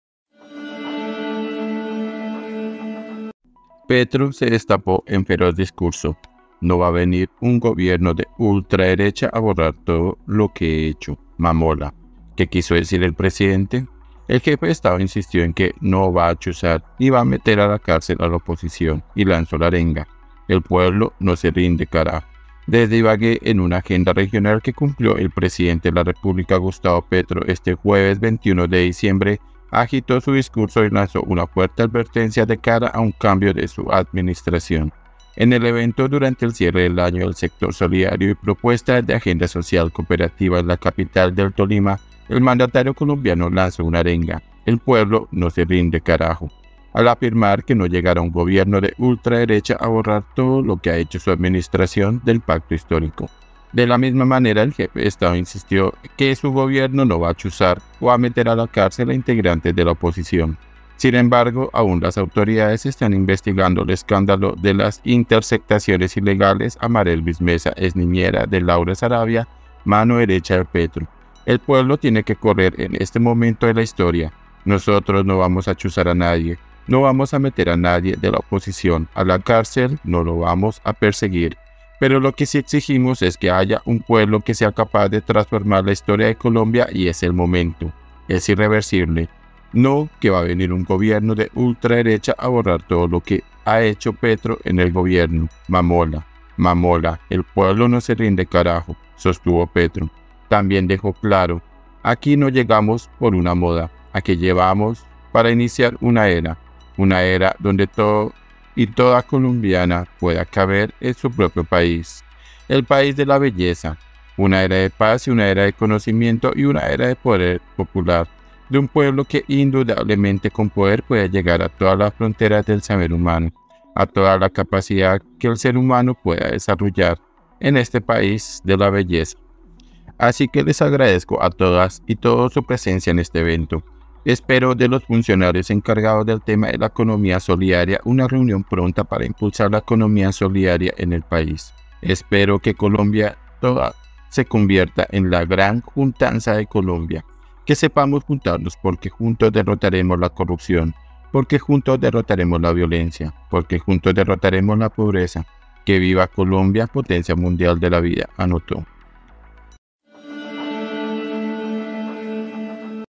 ⚫ PETRO SE DESTAPÓ EN FERÓZ DISCURSO
Petro se destapó en feroz discurso: “No va a venir un gobierno de ultraderecha a borrar todo lo que he hecho, mamola”. ¿Qué quiso decir el presidente?
El jefe de Estado insistió que “no va a chuzar” ni va a “meter” a la cárcel a la oposición. Y lanzó la arenga: “El pueblo no se rinde carajo”.
SEMANA